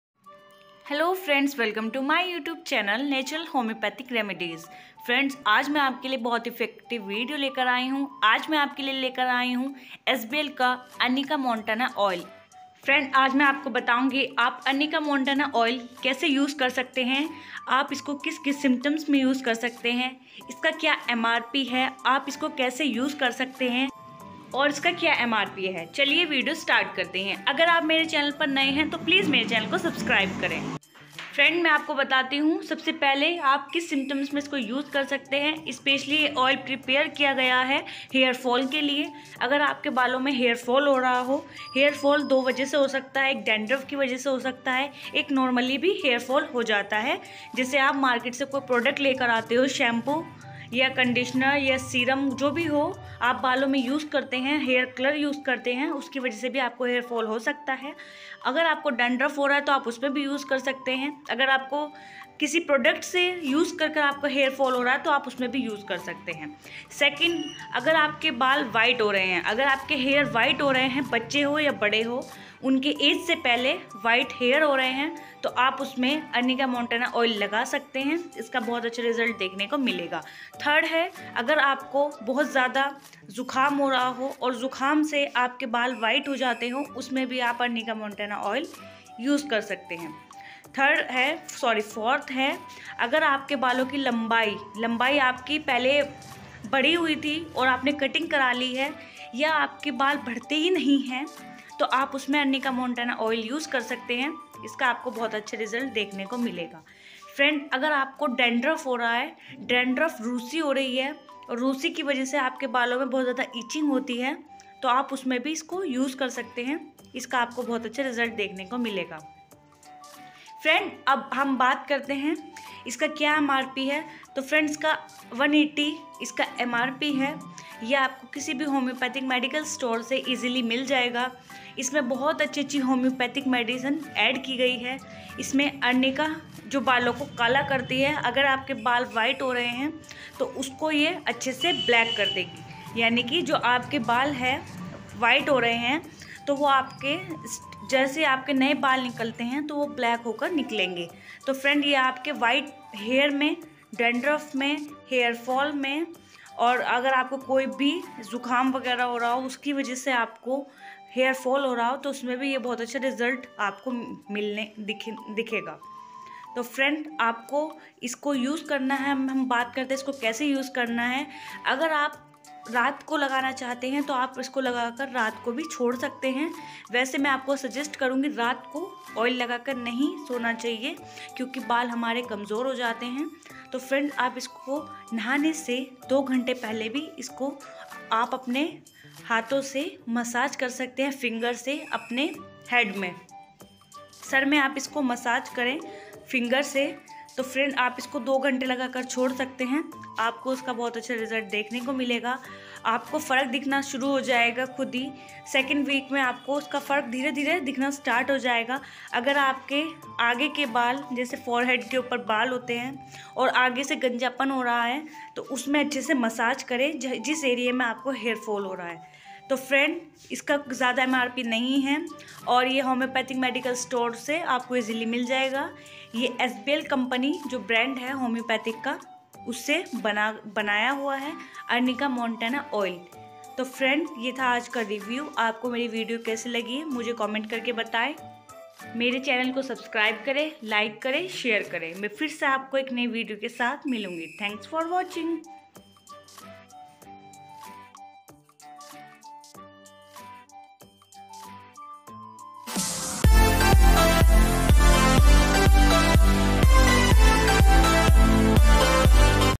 SBL Arnica Montana Hair oil // Arnica Montana oil for Dandruff hairfall / बालो की समस्या करे अब खत्म
दोस्तो आज हमने आर्निका मोंटाना हेयर को कैसे यूज करे और बालों की सभी समस्या को करें खत्म।।
पूरी वीडियो लास्ट तक जरूर देखे।।

thnks for watching my videos